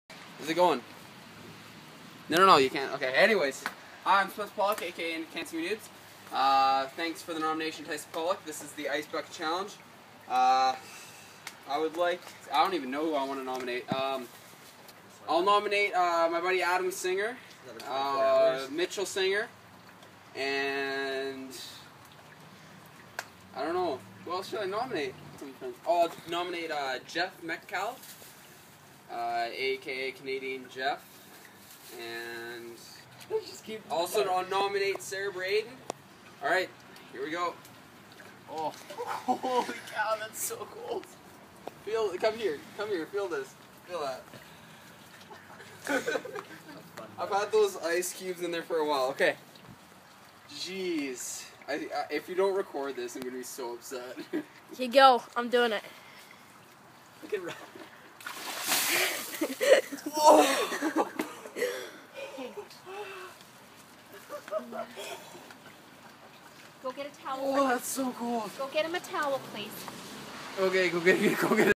ALS Ice Bucket Challenge-cantcmenoobz
Hey all!

Here is my Ice Bucket Challenge.

My brother nominated me, so I guess I would like to thank him.

I nominate Mitchell Singer (@mitchysinger)
Adam Singer (@adam_singer11)
and Sarah Braaten (@sarahhemilie)

You guys all have 24 hours or you have to give $100 (CAN) to the Amyotrophic Lateral Sclerosis (ALS) foundation

Enjoy the video! And good luck reaching your goal ALS foundation! I really hope a cure can be found with any disease, and everyone deserves a fighting chance for their own life. Losing someone to any disease is terrible, and to all those who have lost someone to this disease, my heart goes out to you all.

And what a great idea to utilize social media for your fundraiser ALS foundation. I must say, really well done, and a great way to raise awareness for the disease and to raise some cash for your cause.

(PS I'm at my friends house, so I just had to upload quickly to make my 24 hour time. This was done on my phone then I went on my laptop to quickly edit it. Also it's my new laptop with a different video editor so I wasn't really used to editing it. It was recorded on my phone and for some reason the video editor changed my resolution to 480p instead of the 720p I selected. Just FYI)